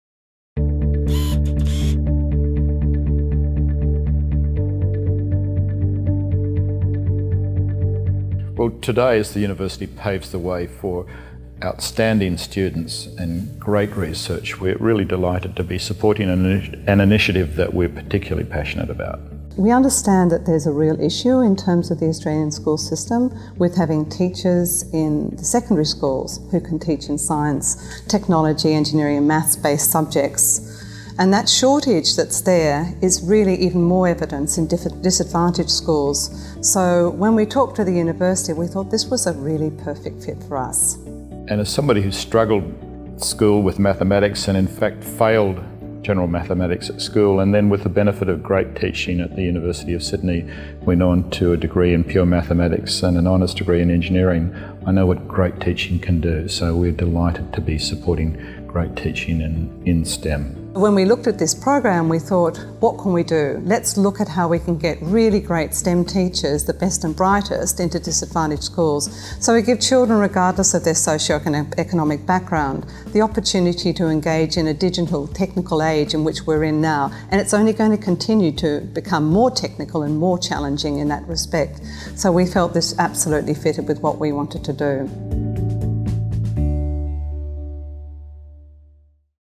Roger Massy-Greene and University of Sydney Chancellor give $1m to support STEM teacher education
Husband and wife, Roger Massy-Greene and University of Sydney Chancellor Belinda Hutchinson AM, have announced a $1 million gift to science, technology, engineering and maths (STEM) teacher education.

The gift aims to increase the number of high quality STEM teachers in disadvantaged schools through the establishment of scholarships to attract and support the best postgraduate teaching students.